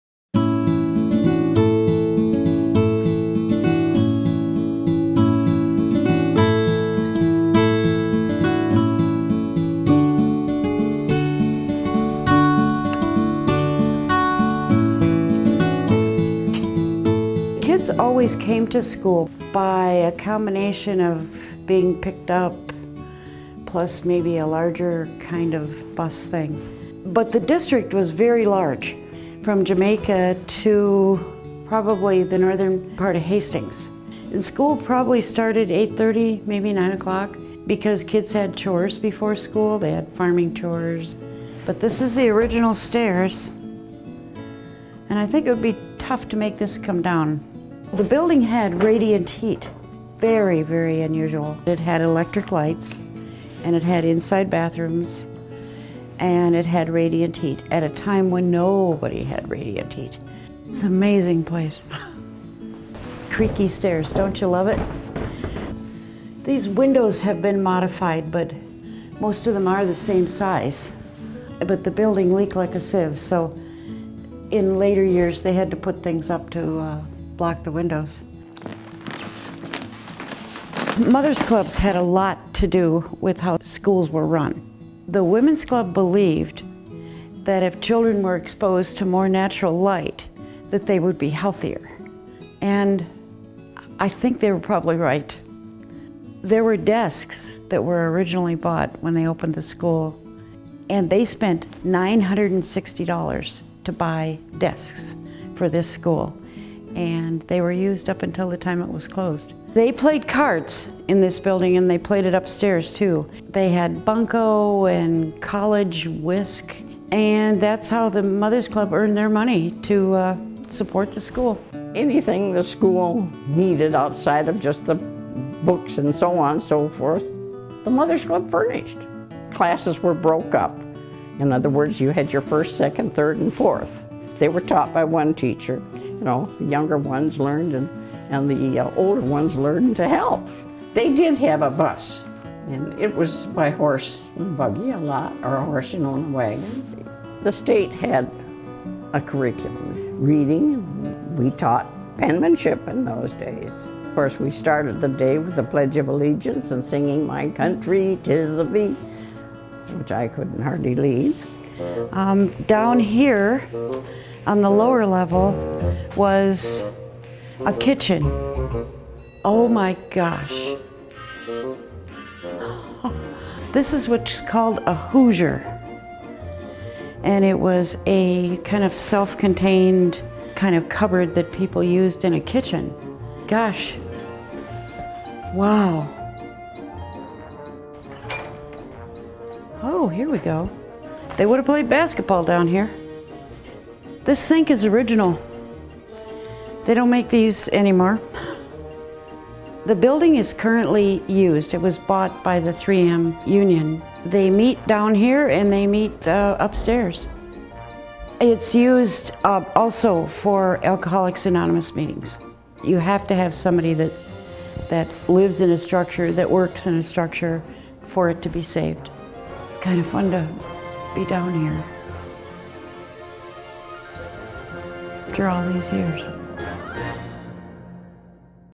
The History of the Langdon School - Cottage Grove
History buff Judy Spooner takes us inside the 2-room school house built in Langdon in 1918. The school building still sits on the west of Cottage Grove.

This video was created by the SWCTC, for more videos about Cottage Grove: